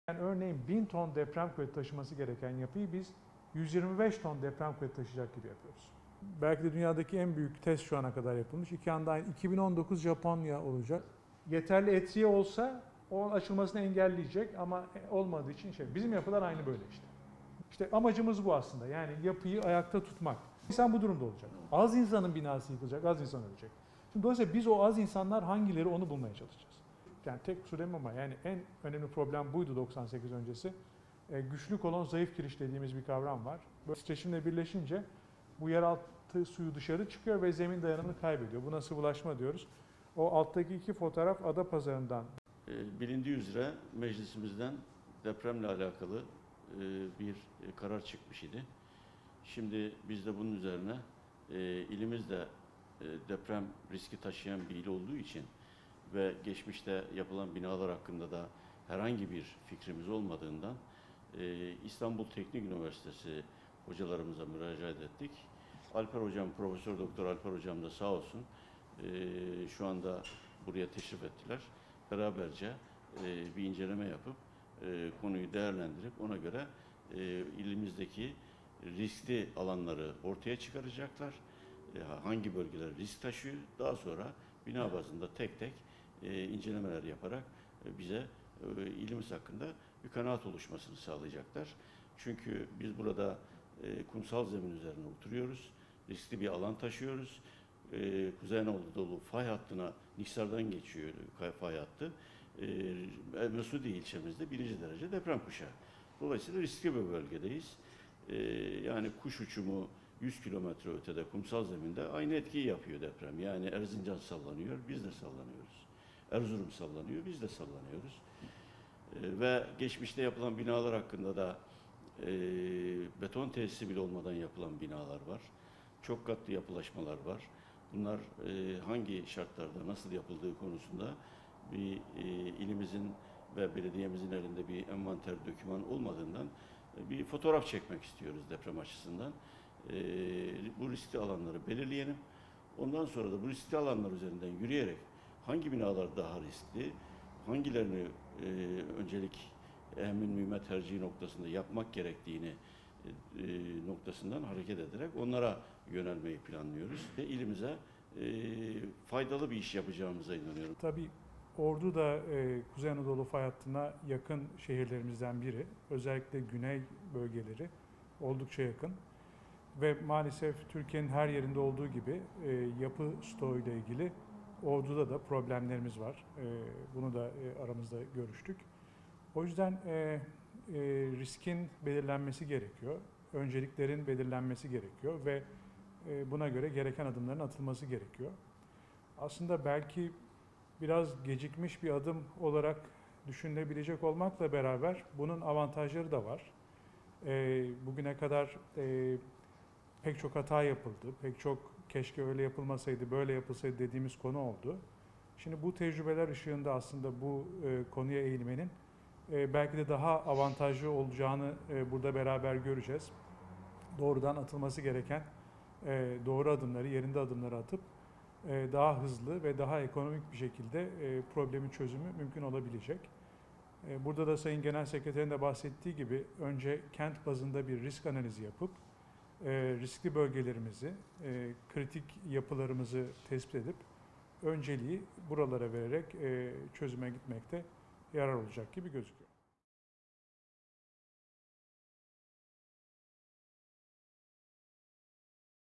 DEPREM MASTER PLANI İÇİN DÜĞMEYE BASILDI
Ordu Büyükşehir Belediye Başkanı Dr. Mehmet Hilmi Güler’in son zamanlarda öne sürdüğü deprem master planı ile ilgili hazırlıklar yapılıyor.
Ordu’daki yapıların geçmişinin bilinmemesi ve ülke genelinde can ve mal kayıplarına sebebiyet veren deprem afetleri sonrası Başkan Güler’in öncülüğünde hazırlanan deprem master planı için düğmeye basıldı.
Bu kapsamda Ordu ili genelinde mevcut yapı rezervlerinin değerlendirilmesi ve incelenmesi için Büyükşehir Belediyesi Genel Sekreteri Coşkun Alp, İstanbul Üniversitesi (İTÜ) İnşaat Fakültesi Öğretim Üyesi Prof. Dr. Alper İlki ve Araştırma Görevlisi Dr. Cem Demir ile bir araya geldi.